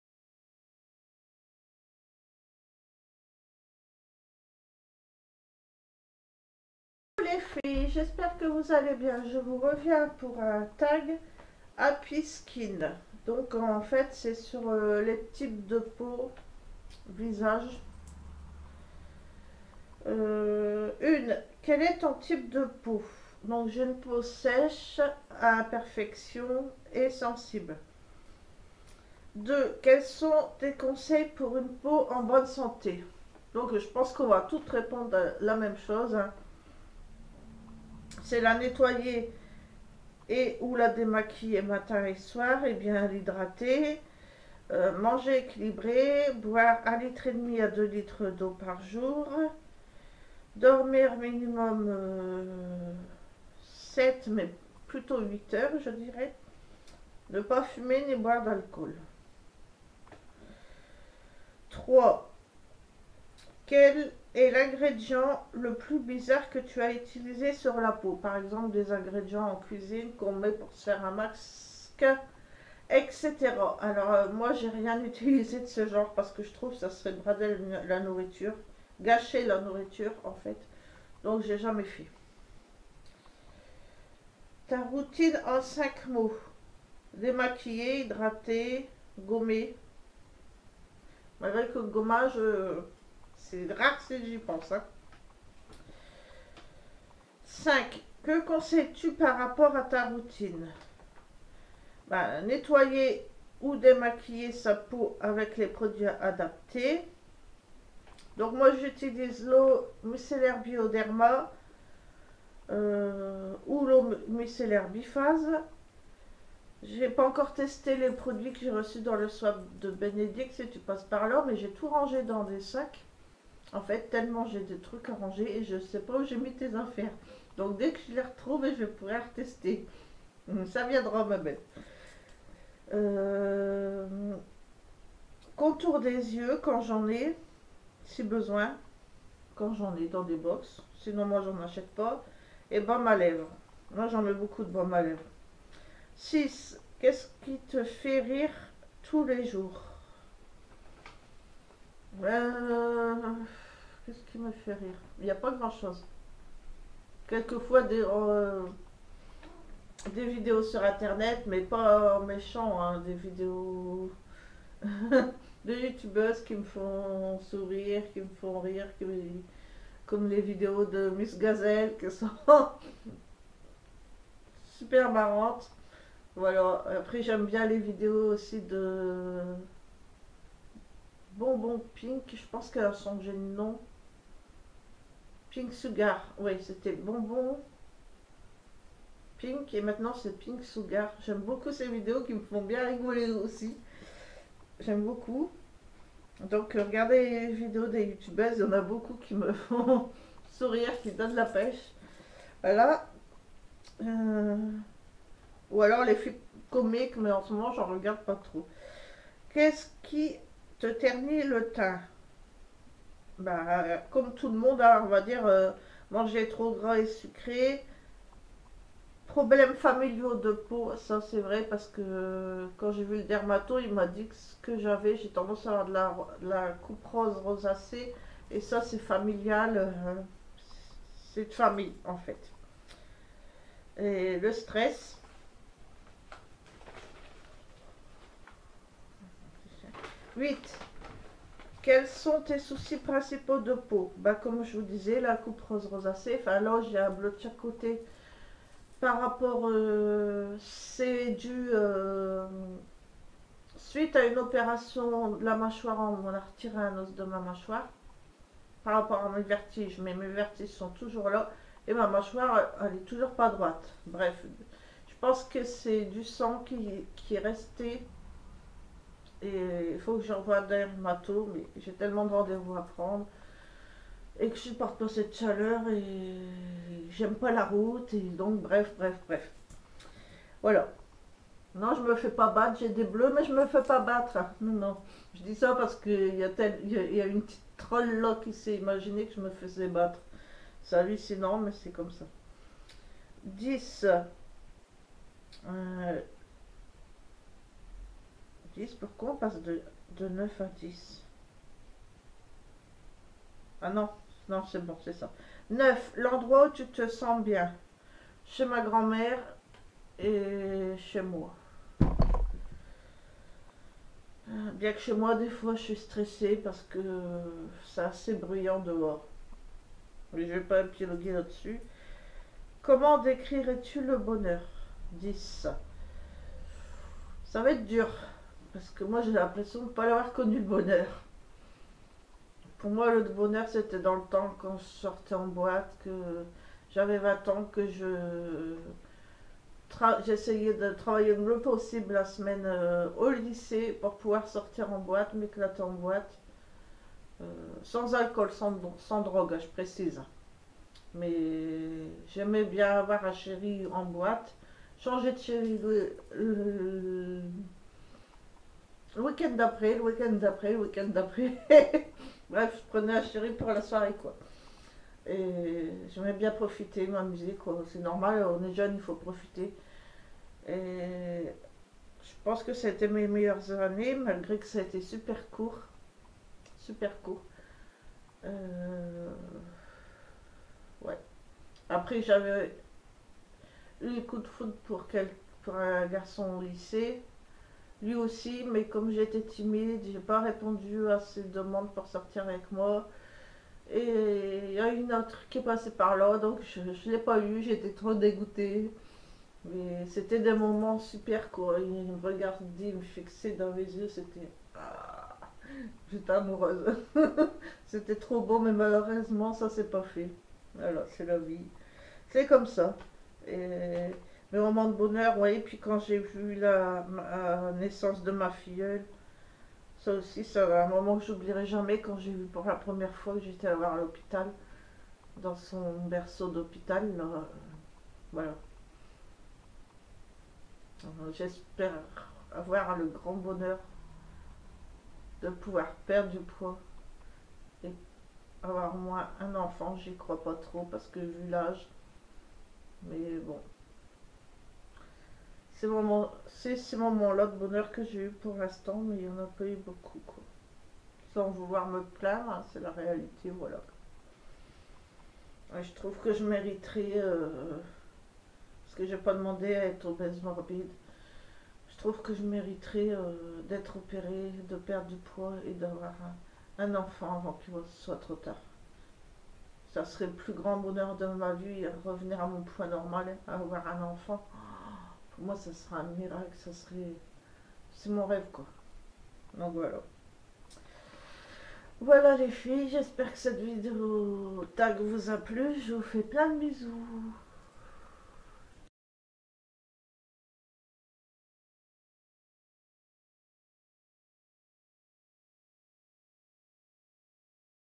Tag : "happy skin" (routine visage)
QUESTIONS

1/ quel est ton type de peau ?
2/ quels sont tes conseils pour une peau en bonne santée ?
3/ quel est l' ingrédient naturel le plus bizarre que tu a utilisé sur ta peau ?
4/ ta routine en 5 mots ?
5/que conseiille tu par rapport a ta routine ?
6/ qu' est ce qui te fait rire tout les jours ?
7/ qu' est ce qui te ternit le teint ?
8/ quels sont tes soucis principaux de peau ?
9/ l' endroit ou tu te sens bien ?
10/ comment décrirais tu le bonheur ?

JE TAGUE :

* Yvetta Asthetik
* Bénédicte de notre vie de femme
* Lorine Snowhite
* Feebl3sse
* Ninette Angely
* Evedereve
* Brunette beautyworld
* Leila Makeup
* Orkid Vanylle
* Jess1308
* Aima Channel
* Nancybeauty
* Nicole tonelle
* Crasy Girl